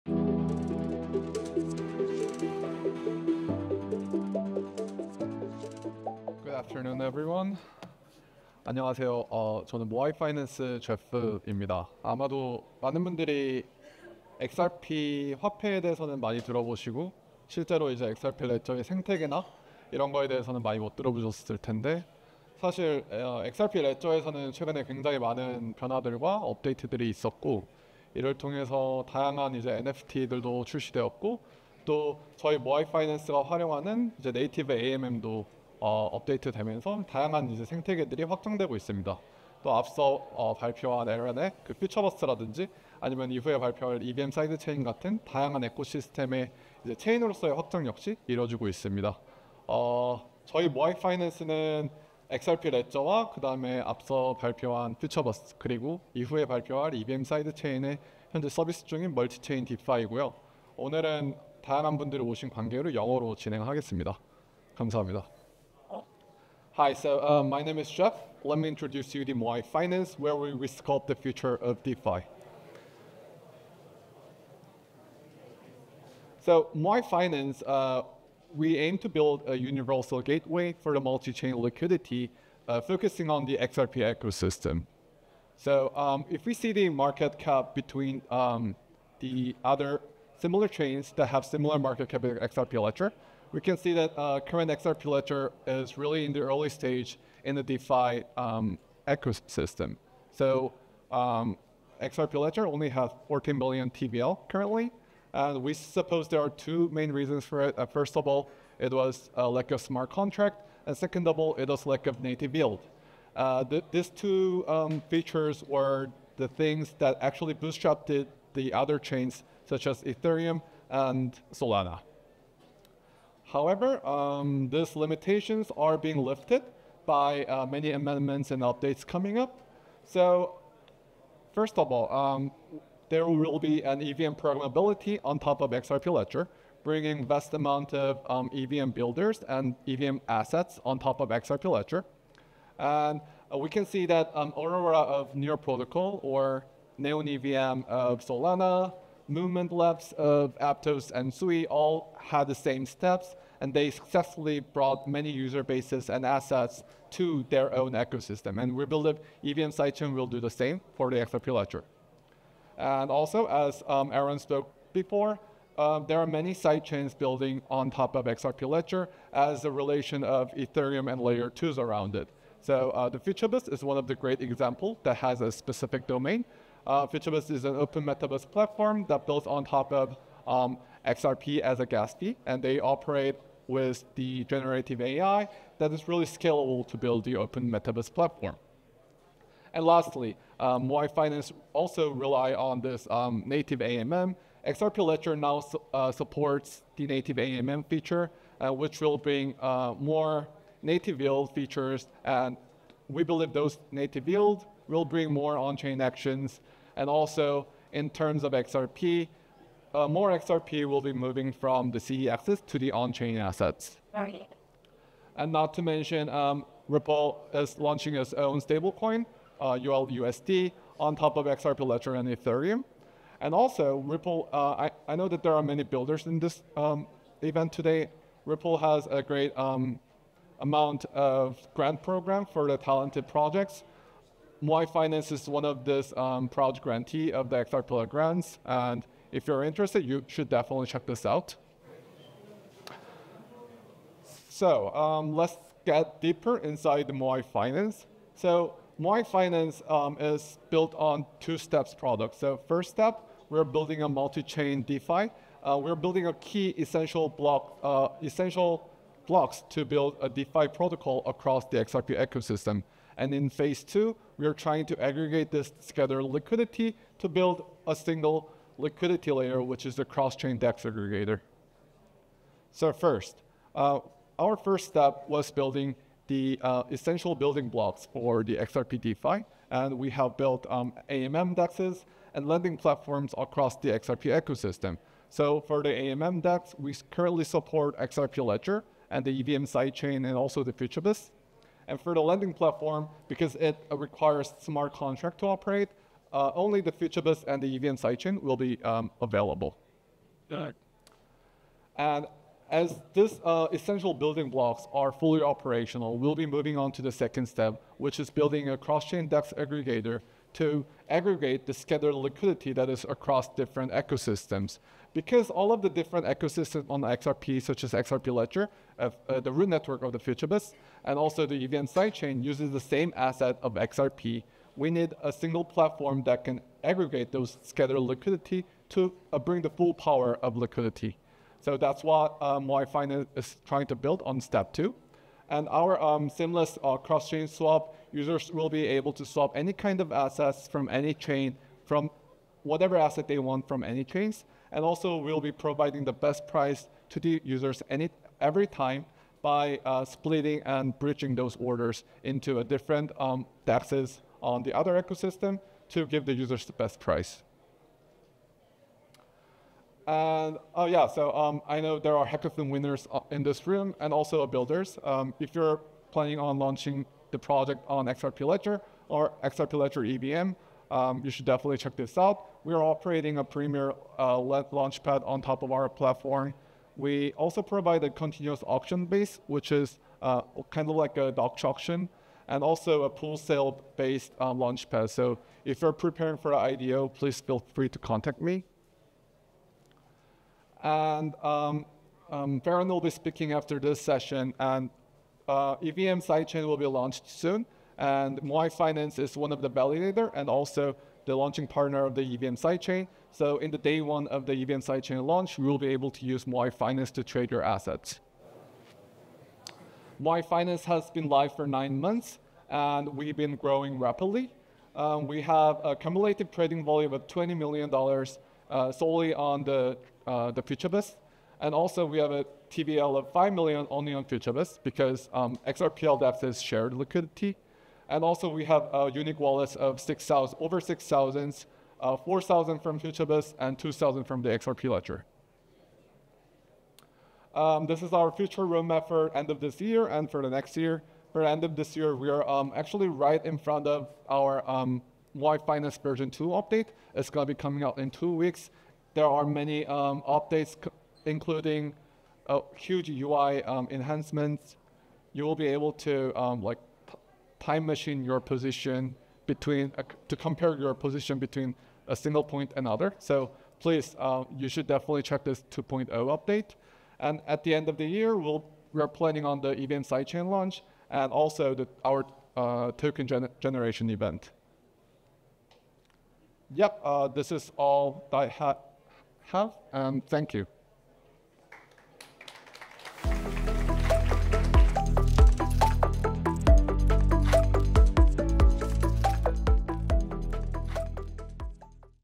Leveraging Moai Finance for AMM Usage
XRPL Zone Seoul brought together Developers, corporates, fintechs, banks, VCs, academia, and the XRP community under one roof for the biggest XRPL event in South Korea!  This special edition of the XRPL Zone focused on key developments on the XRPL including RWA tokenization, the AMM and XRPL EVM Sidechain.